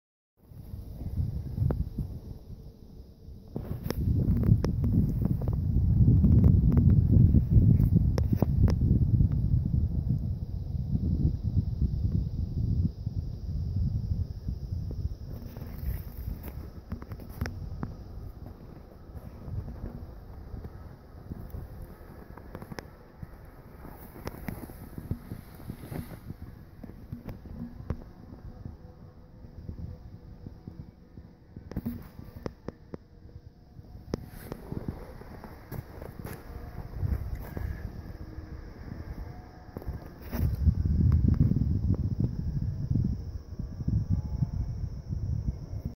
$5 Dollar Snowmobile Scratchers
3" x 5/16" or 3/8" (shown) Clevis pins @ $2 each, washers @ $.08 each. Excellent cooling, hyfax wear.